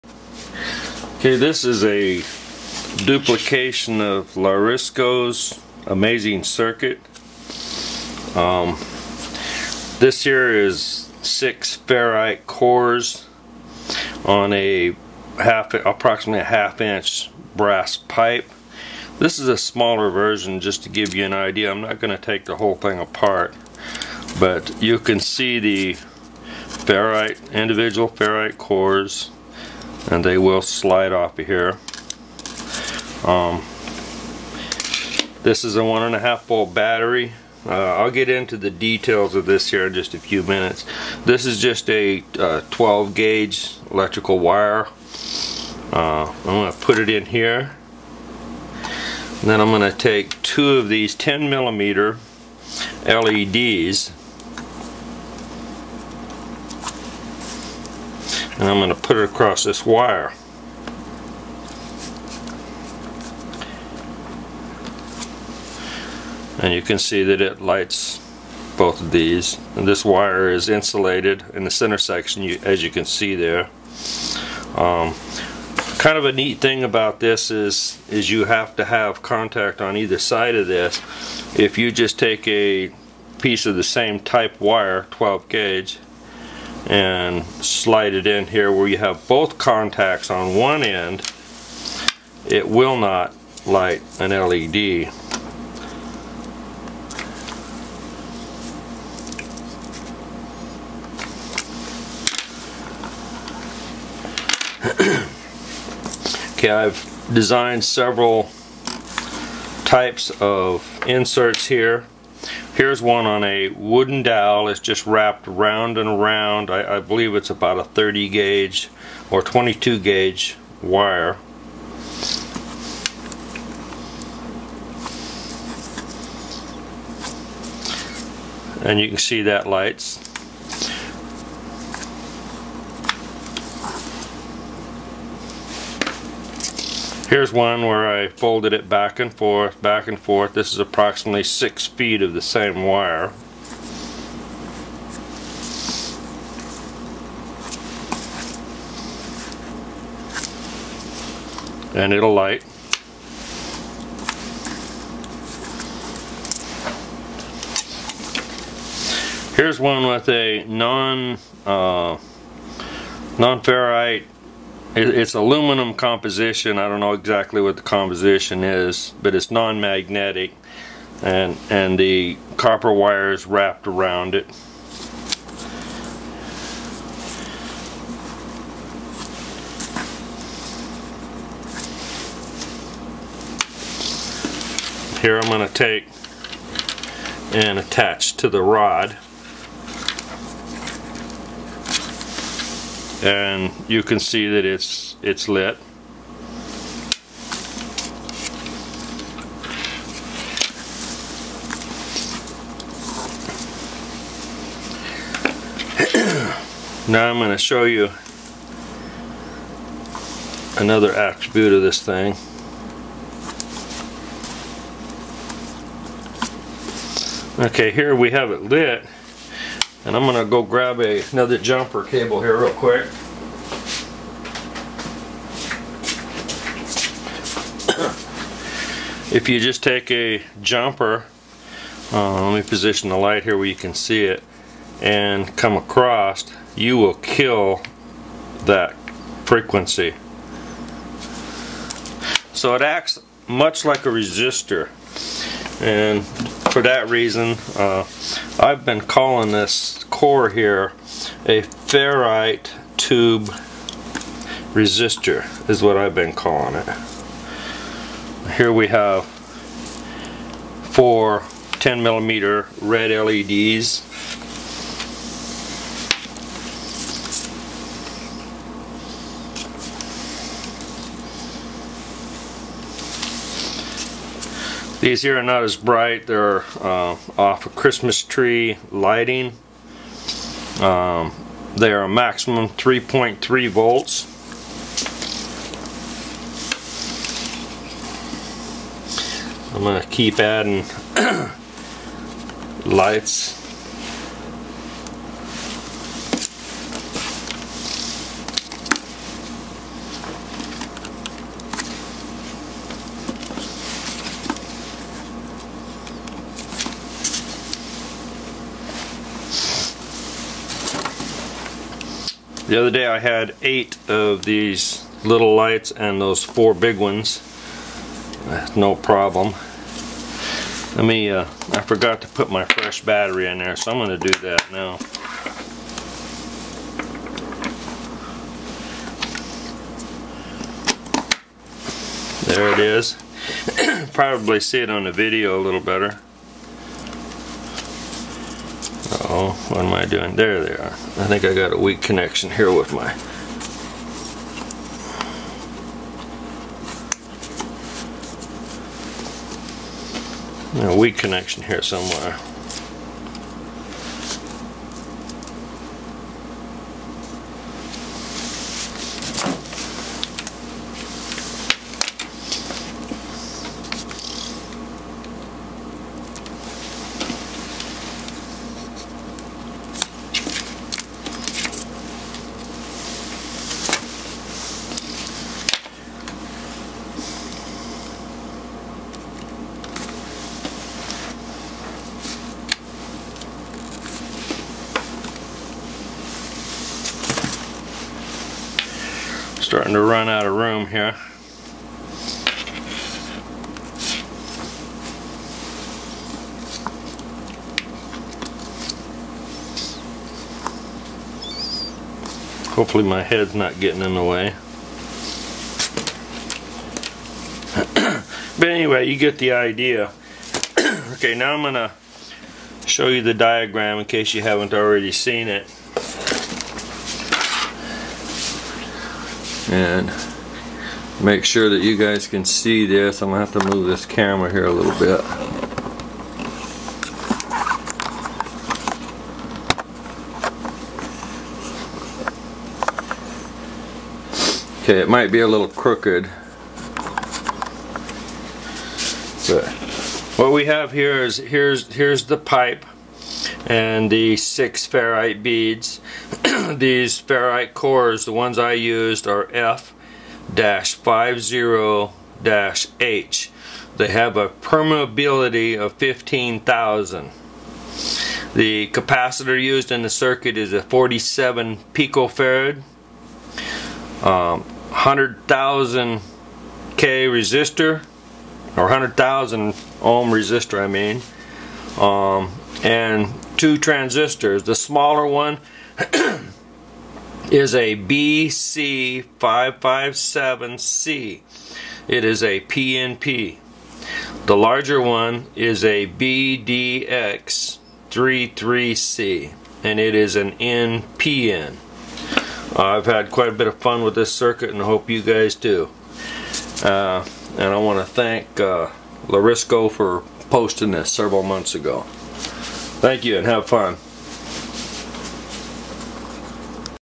Amazing Circuit duplication of Larskro YT-video
This Amazing circuit was first posted on YT by Larskro.  My duplication efforts were rewarded on my 1st attempt!  Thank you Larskro for such great attention to detail in your video.  I just wish other YT'rs would do the same.

   In my video duplication I have taken the liberty to call the the ferrite+ brass/copper pipe a "ferrite tube resistor" (FT-Resistor) because it acts much like a resistor in a circuit..

In my first attempt I used the smaller FT-resistor seen in my video, but quickly built the larger one because it afforded me more room for a variety of different type wire experiments.

 If you place the wire on the out side of the FT-resistor you will get nothing, it only works on the inside.  What I believe is happening here is, when the electromagnetic (DC) pulse collapses in on its self towards the center of the FT-resistor, it generates a much stronger electromagnetic field (frequency)  which allows it to energize the LED(s).  I did try a real small DC motor with no effect to the LED's in the circuit which were lit.  The motor would not run. Nor did it effect the LED's in any way that I could see, unlike the jumper wire that created a dead short killing power to the LED's.

 If you take and jumper across the FT-resistor with a resistor in between, the LED's lit in the circuit will remain lit.  I also had a very small filiment type lite that a single AA battery would just barely lite.  I placed it in the circuit and it lit as well.

 I hope should you decide to build this Amazing circuit that you will have as much fun as I have had.